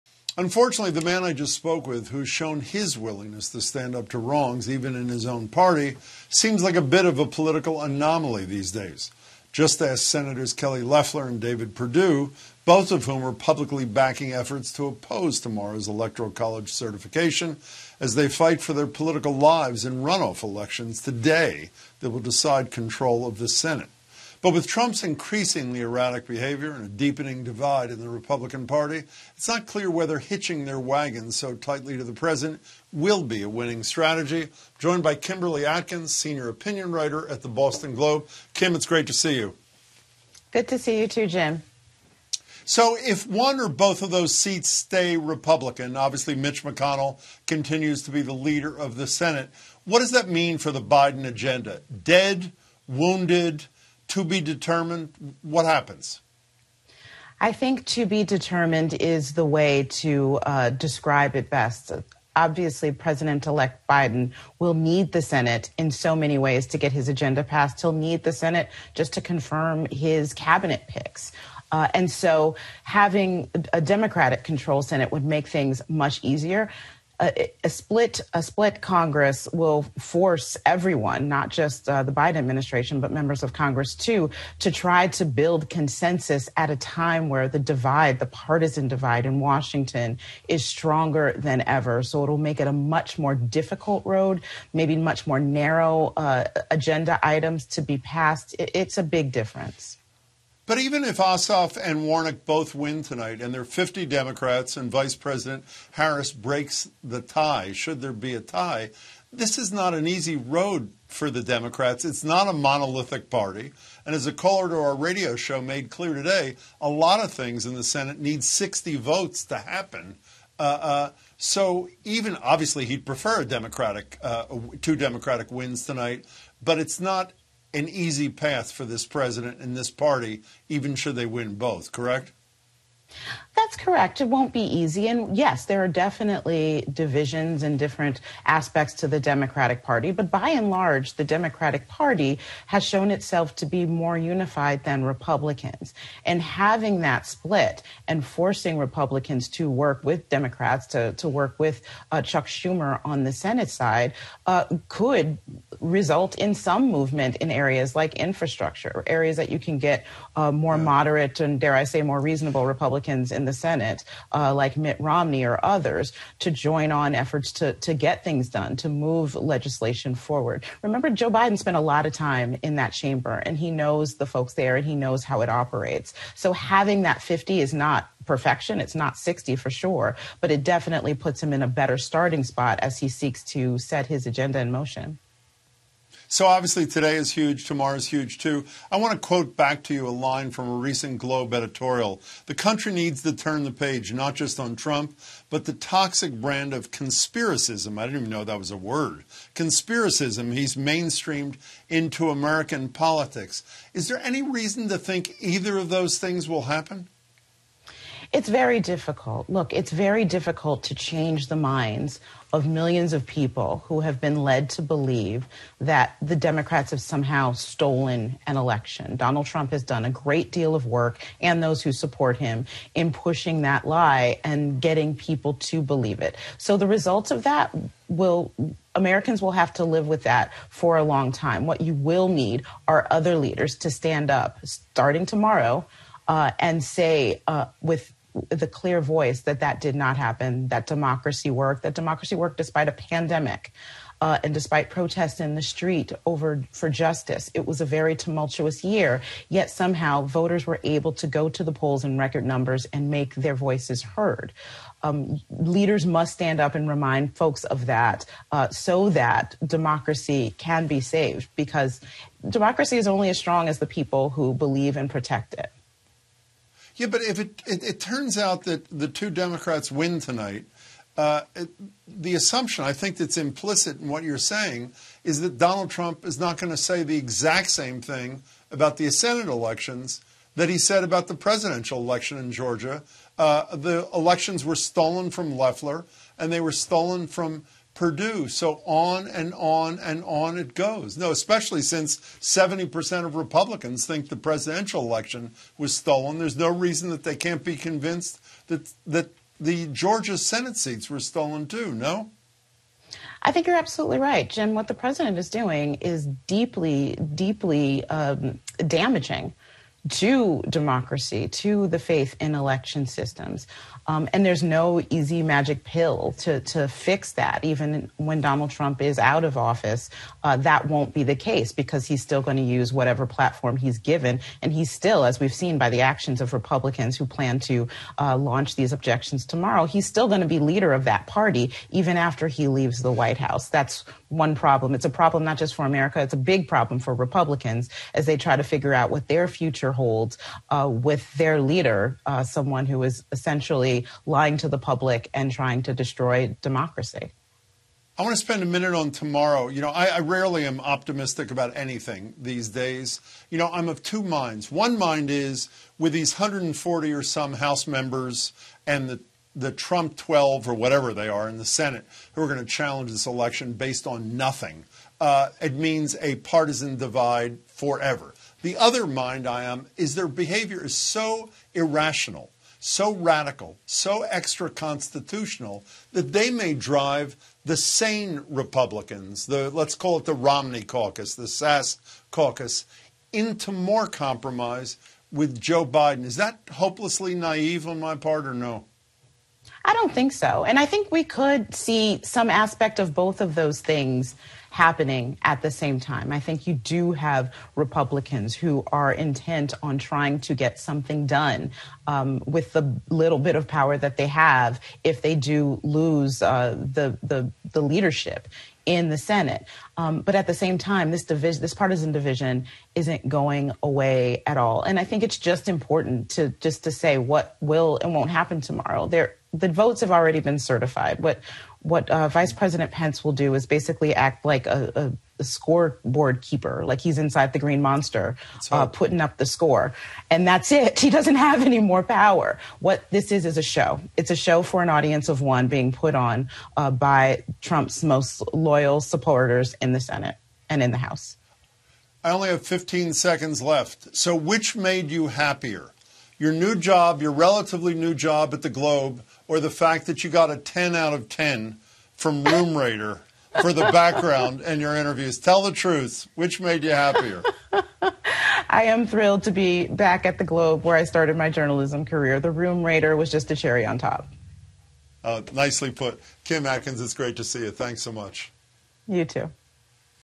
Trump Casts Shadow Over Georgia Senate Runoff Elections
Boston Globe Senior Opinion Writer Kimberly Atkins joined Jim Braude to discuss how heavily President Donald Trump is influencing Georgia politics and the party at large, and what Tuesday’s runoff election results will mean for the Biden administration’s agenda.